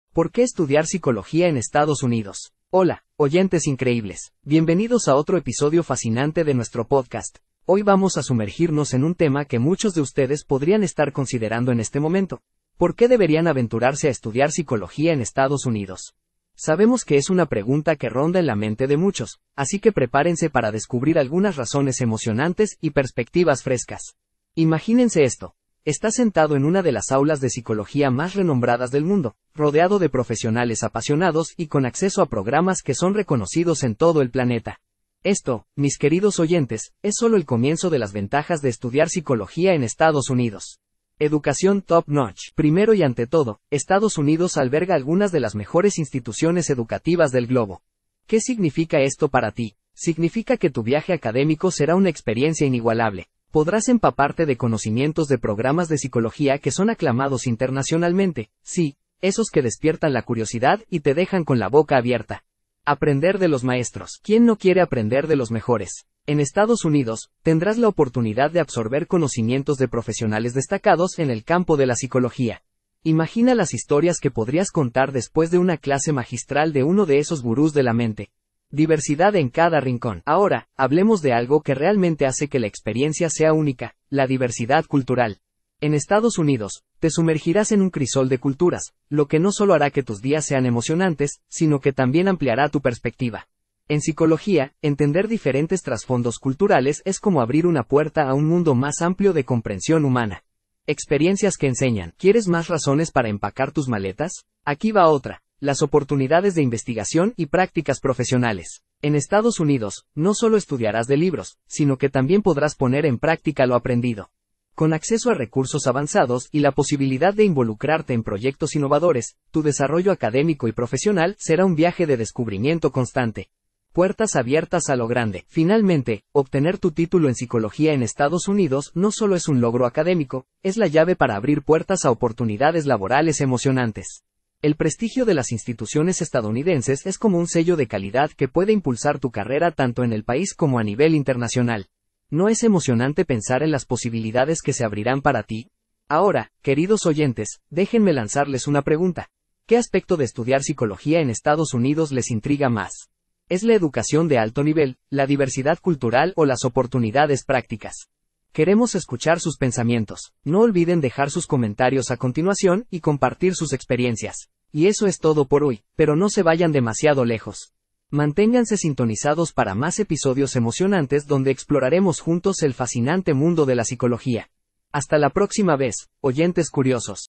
¿Por qué Estudiar Psicología en Estados Unidos?
¡Hola, oyentes increíbles!

¡Bienvenidos a otro episodio fascinante de nuestro podcast! Hoy vamos a sumergirnos en un tema que muchos de ustedes podrían estar considerando en este momento: ¿Por qué deberían aventurarse a estudiar psicología en Estados Unidos? Sabemos que es una pregunta que ronda en la mente de muchos, así que prepárense para descubrir algunas razones emocionantes y perspectivas frescas.

Imagínense esto: estás sentado en una de las aulas de psicología más renombradas del mundo, rodeado de profesionales apasionados y con acceso a programas que son reconocidos en todo el planeta. Esto, mis queridos oyentes, es solo el comienzo de las ventajas de estudiar psicología en Estados Unidos.

Educación Top-Notch:
Primero y ante todo, Estados Unidos alberga algunas de las mejores instituciones educativas del globo. ¿Qué significa esto para ti? Significa que tu viaje académico será una experiencia inigualable. Podrás empaparte de conocimientos de programas de psicología que son aclamados internacionalmente, ¡sí, esos que despiertan la curiosidad y te dejan con la boca abierta!

Aprender de los Maestros:
¿Quién no quiere aprender de los mejores? En Estados Unidos, tendrás la oportunidad de absorber conocimientos de profesionales destacados en el campo de la psicología. ¡Imagina las historias que podrías contar después de una clase magistral de uno de esos gurús de la mente!

Diversidad en Cada Rincón:
Ahora, hablemos de algo que realmente hace que la experiencia sea única: la diversidad cultural. En Estados Unidos, te sumergirás en un crisol de culturas, lo que no solo hará que tus días sean emocionantes, sino que también ampliará tu perspectiva. En psicología, entender diferentes trasfondos culturales es como abrir una puerta a un mundo más amplio de comprensión humana.

Experiencias que Enseñan:
¿Quieres más razones para empacar tus maletas? Aquí va otra: las oportunidades de investigación y prácticas profesionales. En Estados Unidos, no solo estudiarás de libros, sino que también podrás poner en práctica lo aprendido. Con acceso a recursos avanzados y la posibilidad de involucrarte en proyectos innovadores, tu desarrollo académico y profesional será un viaje de descubrimiento constante.

Puertas Abiertas a lo Grande:
Finalmente, obtener tu título en psicología en Estados Unidos no solo es un logro académico, es la llave para abrir puertas a oportunidades laborales emocionantes. El prestigio de las instituciones estadounidenses es como un sello de calidad que puede impulsar tu carrera tanto en el país como a nivel internacional. ¿No es emocionante pensar en las posibilidades que se abrirán para ti?

Ahora, queridos oyentes, déjenme lanzarles una pregunta: ¿Qué aspecto de estudiar psicología en Estados Unidos les intriga más? ¿Es la educación de alto nivel, la diversidad cultural o las oportunidades prácticas? ¡Queremos escuchar sus pensamientos! No olviden dejar sus comentarios a continuación y compartir sus experiencias.

Y eso es todo por hoy, pero no se vayan demasiado lejos. Manténganse sintonizados para más episodios emocionantes donde exploraremos juntos el fascinante mundo de la psicología. ¡Hasta la próxima vez, oyentes curiosos!